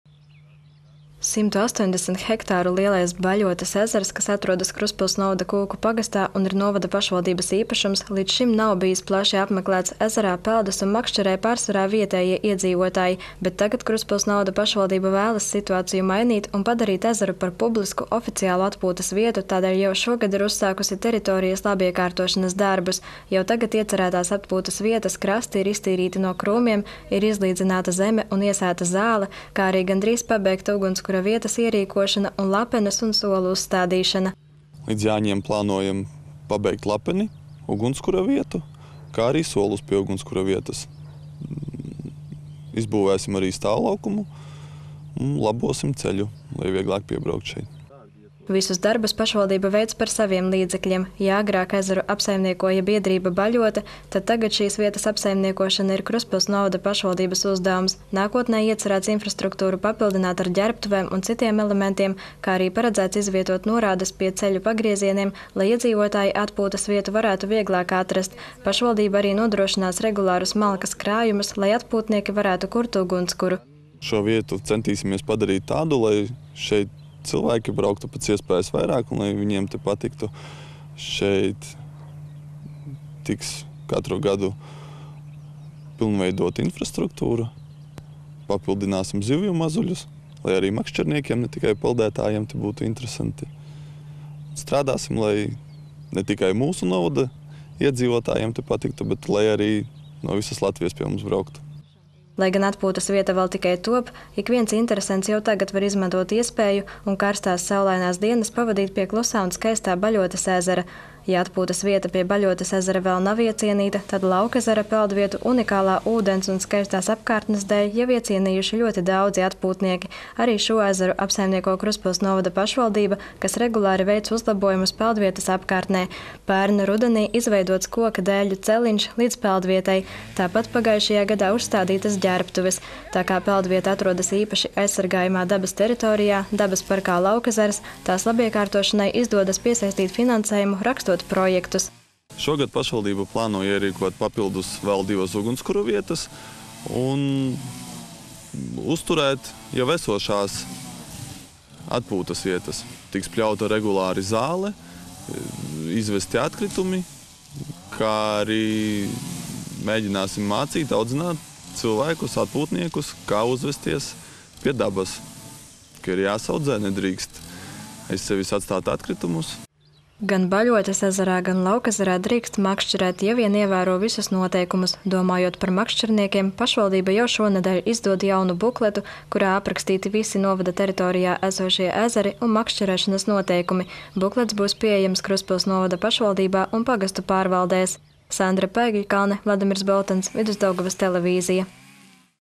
Krustpils novadā notiek Baļotes ezera un Laukezera labiekārtošana
180 hektāru lielais Baļotes ezers, kas atrodas Krustpils novada Kūku pagastā un ir novada pašvaldības īpašums, līdz šim nav bijis plaši apmeklēts -- ezerā peldas un makšķerē pārsvarā vietējie iedzīvotāji. Bet tagad Krustpils novada pašvaldība vēlas situāciju mainīt un padarīt ezeru par publisku, oficiālu atpūtas vietu, tādēļ jau šogad ir uzsākusi teritorijas labiekārtošanas darbus.
    Jau tagad iecerētās atpūtas vietas krasti ir iztīrīti no krūmiem, ir izlīdzināta zeme un iesēta zāle, kā arī gandrīz pabeigta ugunskura vietas ierīkošana un lapenes un solu uzstādīšana.
    Krustpils novada pašvaldības Vides un civilās aizsardzības dienesta vadītājs Ilmārs Luksts stāsta: „Līdz Jāņiem plānojam pabeigt lapeni, ugunskura vietu, ka arī solus pie ugunskura vietas. Izbūvēsim arī stāvlaukumu un labosim ceļu, lai vieglāk šeit piebraukt."

    Visus darbus pašvaldība veic par saviem līdzekļiem. Ja agrāk ezeru apsaimniekoja biedrība „Baļote", tad tagad šīs vietas apsaimniekošana ir Krustpils novada pašvaldības uzdevums. Nākotnē iecerēts infrastruktūru papildināt ar ģērbtuvēm un citiem elementiem, kā arī paredzēts izvietot norādes pie ceļu pagriezieniem, lai iedzīvotāji atpūtas vietu varētu vieglāk atrast. Pašvaldība arī nodrošinās regulārus malkas krājumus, lai atpūtnieki varētu kurt ugunskuru.
    Ilmārs Luksts stāsta: „Šo vietu centīsimies padarīt tādu, lai šeit brauktu pēc iespējas vairāk cilvēku, lai viņiem te patiktu. Katru gadu tiks pilnveidota infrastruktūra. Papildināsim ezeru ar zivju mazuļiem, lai arī makšķerniekiem, ne tikai peldētājiem te būtu interesanti. Strādāsim, lai ne tikai mūsu novada iedzīvotājiem te patiktu, bet arī lai no visas Latvijas pie mums brauktu."

    Lai gan atpūtas vieta vēl tikai top, ikviens interesents jau tagad var izmantot iespēju un karstās, saulainās dienas pavadīt pie klusā un skaistā Baļotes ezera.

    Ja atpūtas vieta pie Baļotes ezera vēl nav iecienīta, tad Laukezera peldvietu unikālā ūdens un skaistās apkārtnes dēļ jau iecienījuši ļoti daudzi atpūtnieki. Arī šo ezeru apsaimnieko Krustpils novada pašvaldība, kas regulāri veic uzlabojumus peldvietas apkārtnē. Pērn rudenī izveidots koka dēļu celiņš līdz peldvietai, tāpat pagājušajā gadā uzstādītas ģērbtuves. Tā kā peldvieta atrodas īpaši aizsargājamā dabas teritorijā -- dabas parkā „Laukezers", tās labiekārtošanai izdodas piesaistīt finansējumu, rakstot projektus.

    „Šogad pašvaldība plāno ierīkot vēl papildu divas ugunskura vietas un uzturēt jau esošās atpūtas vietas. Regulāri tiks pļauta zāle un izvesti atkritumi. Tāpat mēģināsim mācīt, audzināt atpūtniekus, kā uzvesties pie dabas. Atgādināsim, ka ir jāsaudzē, ka nedrīkst aiz sevis atstāt atkritumus," skaidro Ilmārs Luksts.

    Gan Baļotes ezerā, gan Laukezerā drīkst makšķerēt, ja vien ievēro visus noteikumus. Domājot par makšķerniekiem, pašvaldība jau šonedēļ izdod jaunu bukletu, kurā aprakstīti visi novada teritorijā esošie ezeri un makšķerēšanas noteikumi. Buklets būs pieejams Krustpils novada pašvaldībā un pagastu pārvaldēs.

    Sandra Paegļkalne